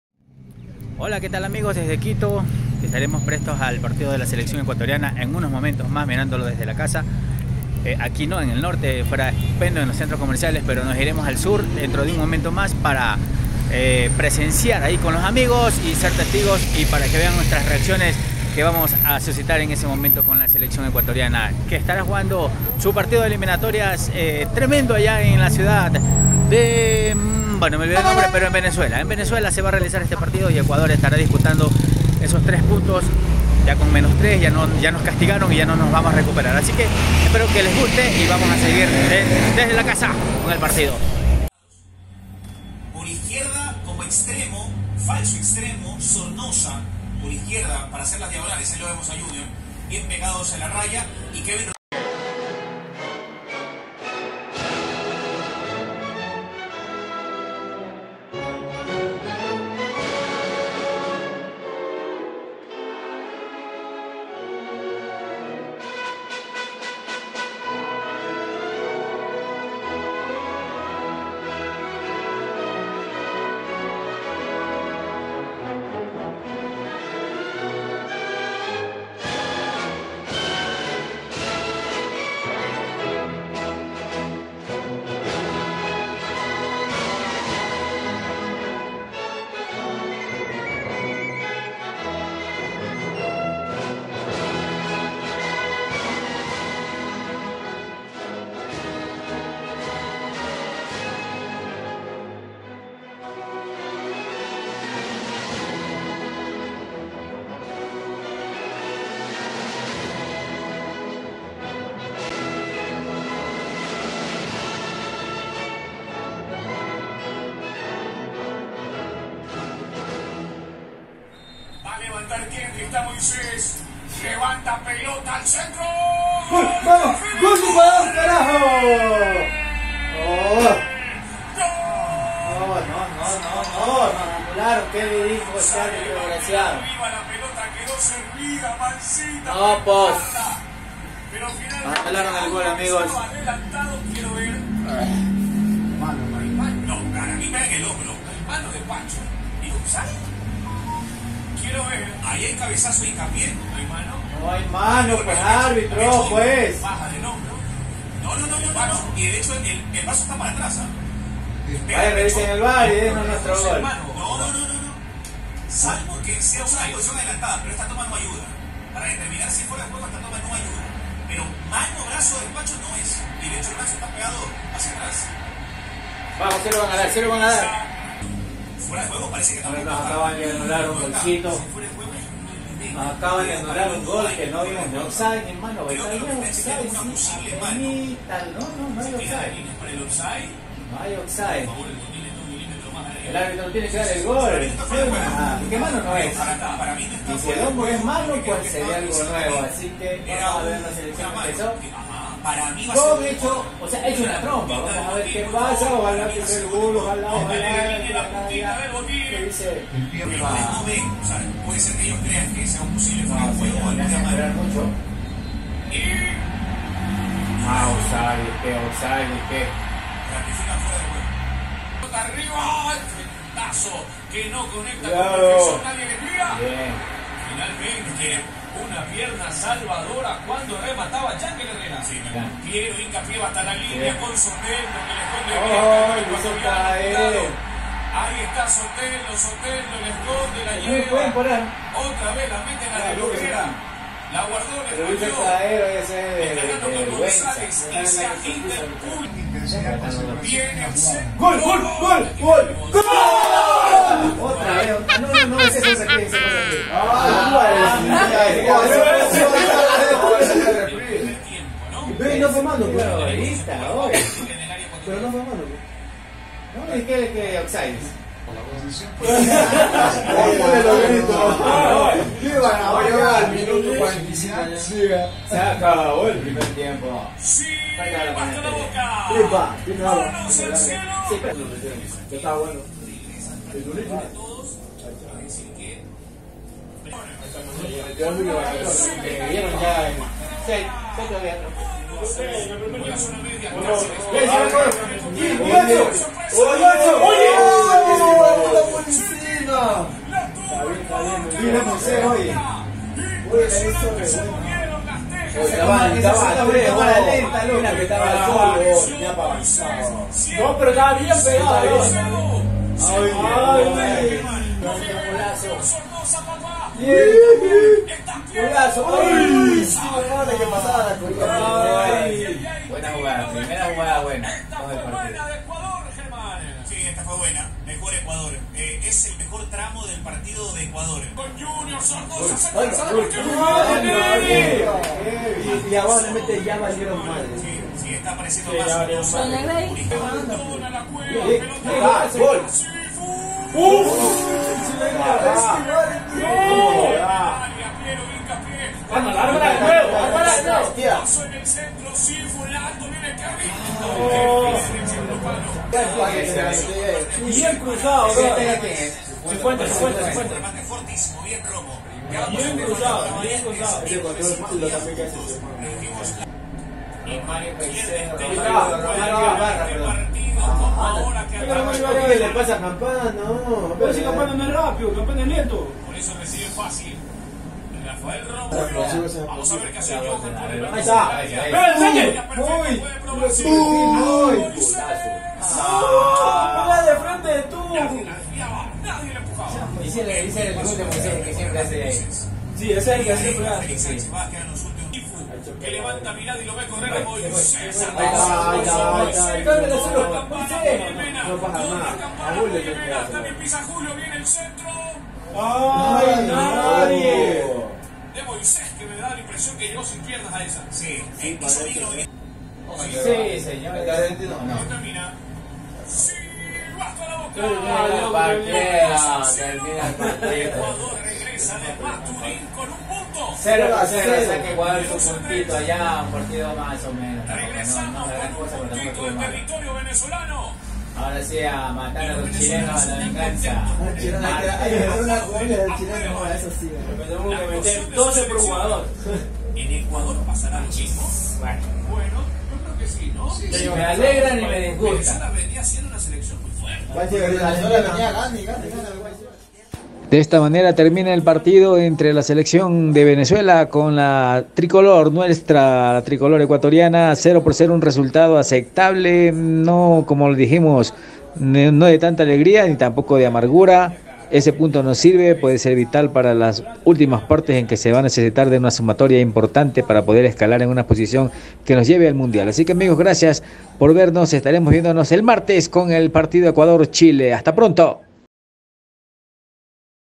¡EMPATE VALIOSO DE LA TRICOLOR! 🤩⚽️ELIMINATORIAS EN MATURIN!
Venezuela 0 - 0 Ecuador
Venezuela 🇻🇪 0-0 Ecuador 🇪🇨 en el estadio Monumental de Maturín